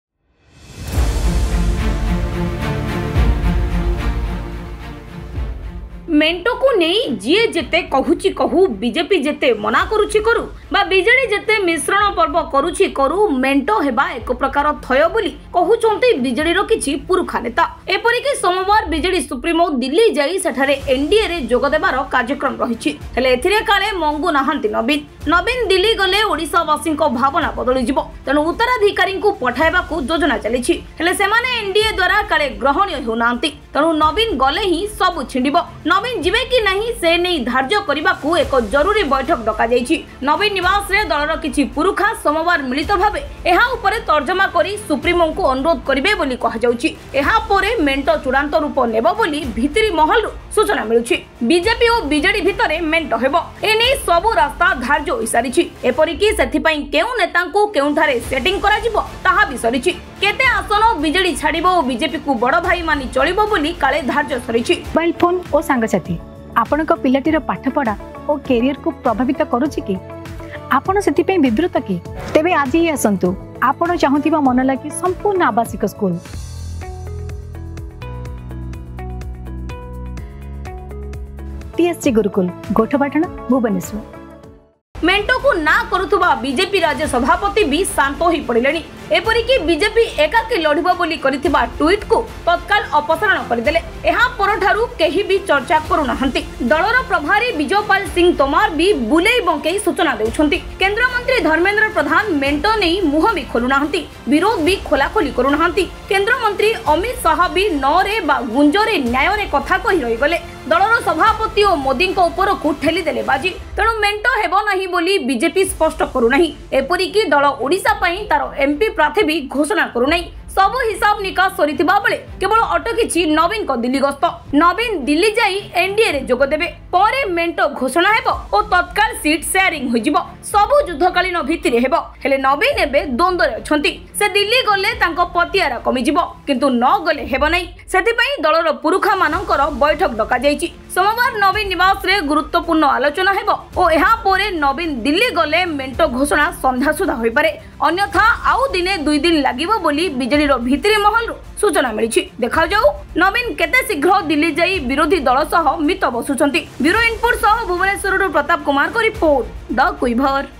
Meeting at Navin Nivas to find the right way | ରାସ୍ତା ଖୋଜିବାକୁ ନିବାସରେ ବୈଠକ
ମେ କୁ ନେଇ ଯିଏ ଯେତେ କହୁଛି କହୁ, ବିଜେପି ଯେତେ ମନା କରୁଛି କରୁ ବା ବିଜେଡି ଯେତେ ମିଶ୍ରଣ ପର୍ବ କରୁଛି କରୁ ମେ ହେବା ଏକ ପ୍ରକାର ଥୟ ବୋଲି କହୁଛନ୍ତି ବିଜେଡିର କିଛି ପୁରୁଖା ନେତା । ଏପରିକି ସୋମବାର ବିଜେଡି ସୁପ୍ରିମୋ ଦିଲ୍ଲୀ ଯାଇ ସେଠାରେ ଏନଡିଏରେ ଯୋଗଦେବାର କାର୍ଯ୍ୟକ୍ରମ ରହିଛି । ହେଲେ ଏଥିରେ କାଳେ ମଙ୍ଗୁ ନାହାନ୍ତି ନବୀନ । ନବୀନ ଦିଲ୍ଲୀ ଗଲେ ଓଡ଼ିଶା ବାସୀଙ୍କ ଭାବନା ବଦଳିଯିବ । ତେଣୁ ଉ ରାଧିକାରୀଙ୍କୁ ପଠାଇବାକୁ ଯୋଜନା ଚାଲିଛି । ହେଲେ ସେମାନେ ଏନଡିଏ ଦ୍ୱାରା କାଳେ ଗ୍ରହଣୀୟ ହେଉନାହାନ୍ତି । ତେଣୁ ନବୀନ ଗଲେ ହିଁ ସବୁ ଛିଣ୍ଡିବ ।